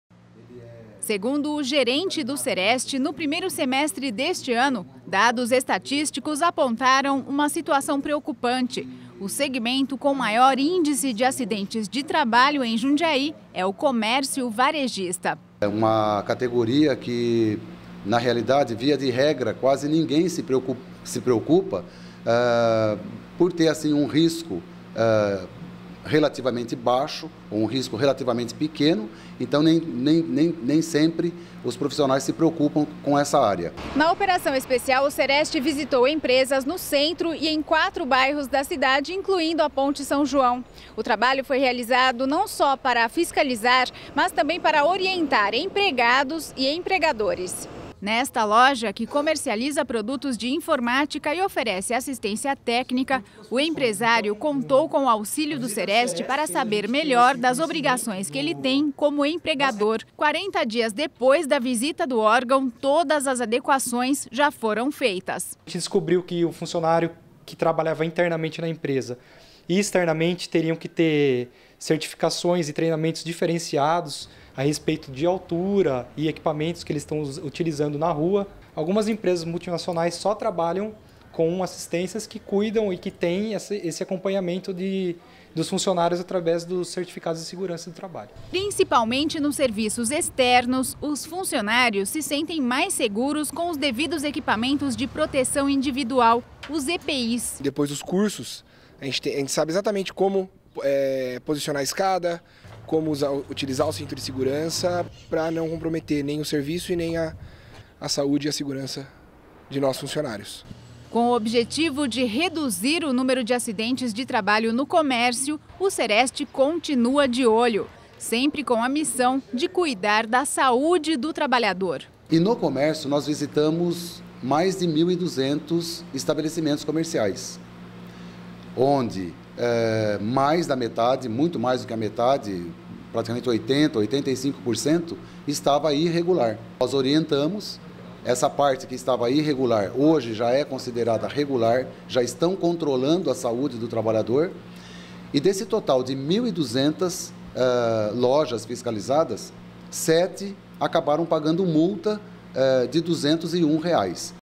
CEREST fiscaliza a segurança de mais de mil empresas em 2014
O CEREST, Centro de Referência em Saúde do Trabalhador, fiscalizou mais de mil empresas em 2014. E um dado surpreendeu o órgão: ao invés das indústrias, o comércio varejista foi o segmento que apresentou maior índice de acidentes de trabalho em Jundiaí.